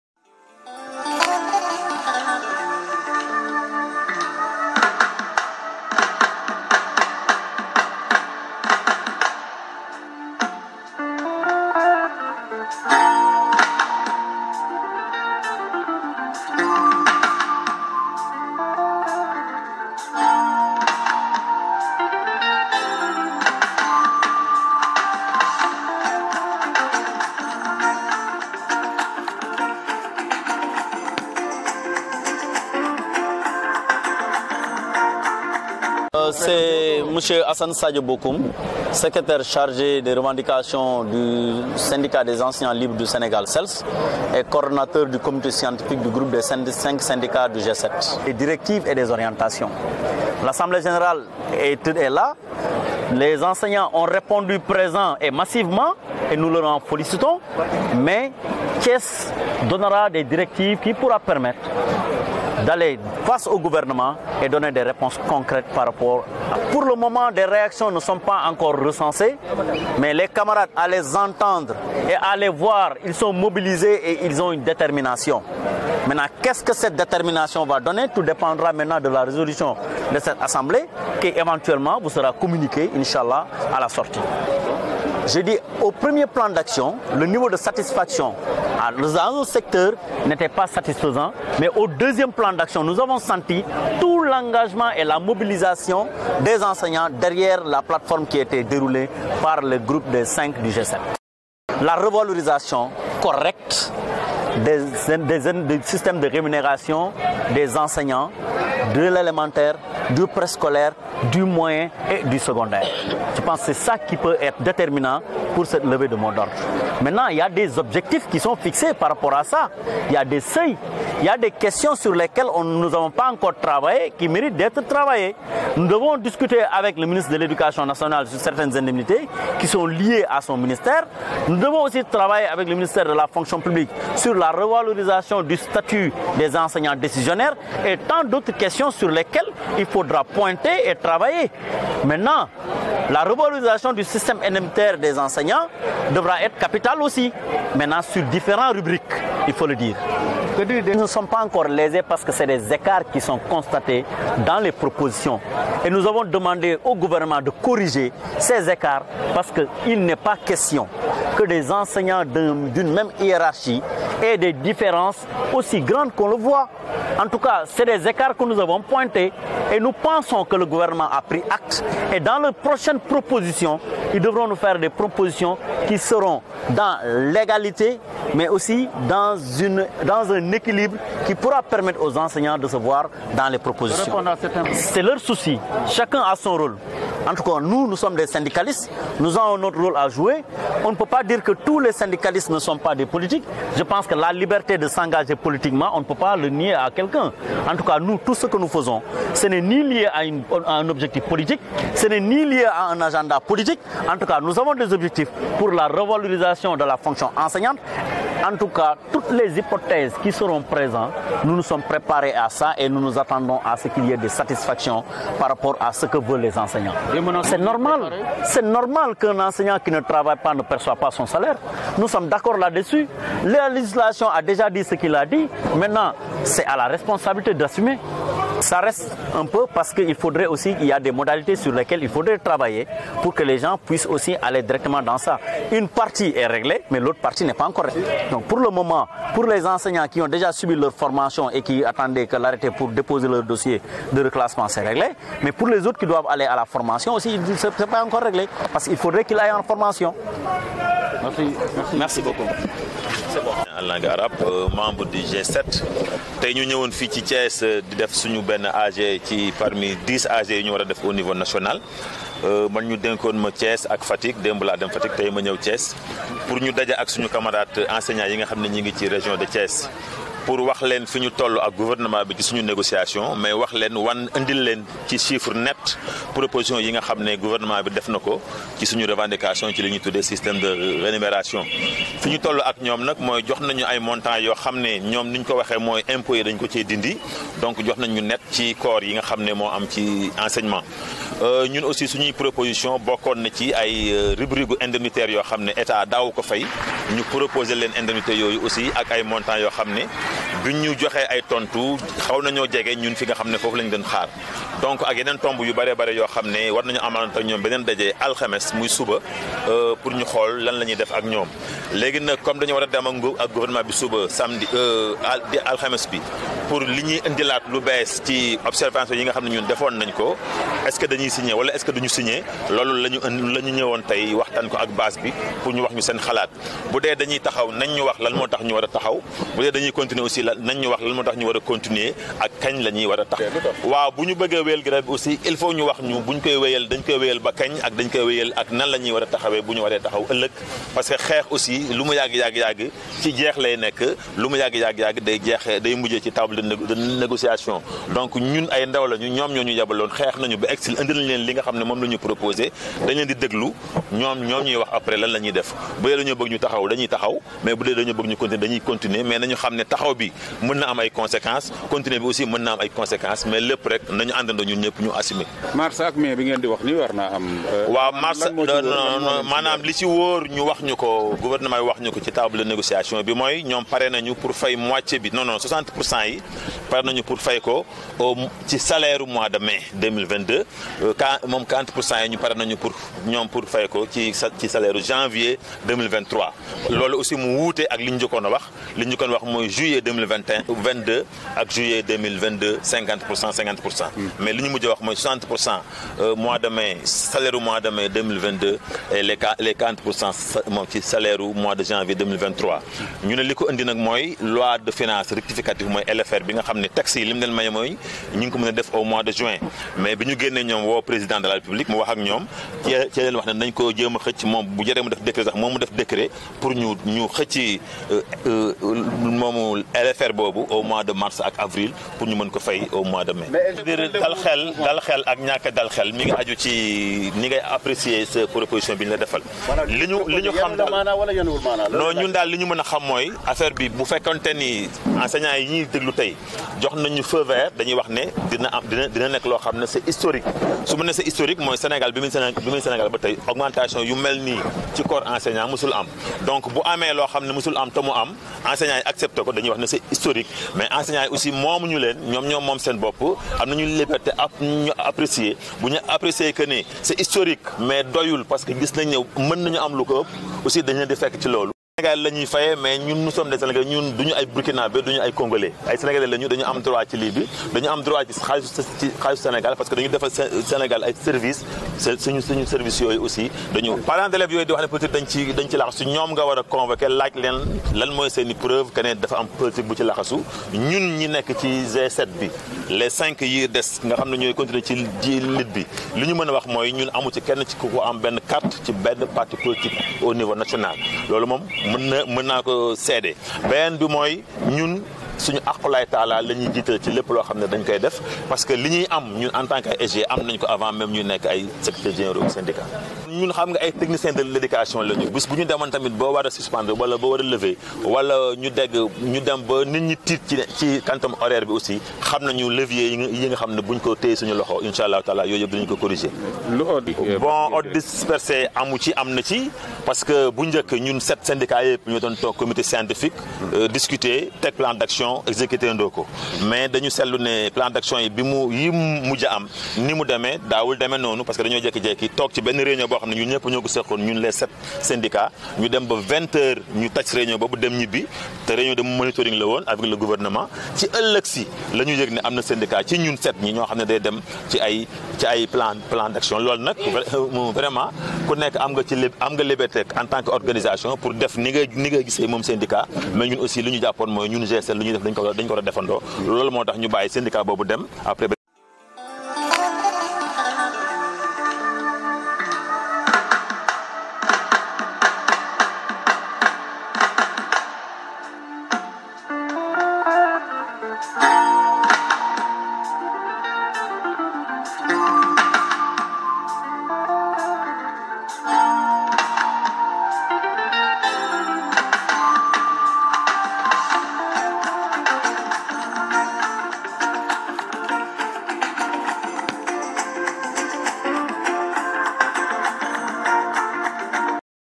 A G DES ENSEIGNANTS: L'Etat à petits pas vers un respect des doléances.
Vraies Nouvelles dans le Web /Deugue Reke
(Par Frédéric DIALLO & Thierno CISSE)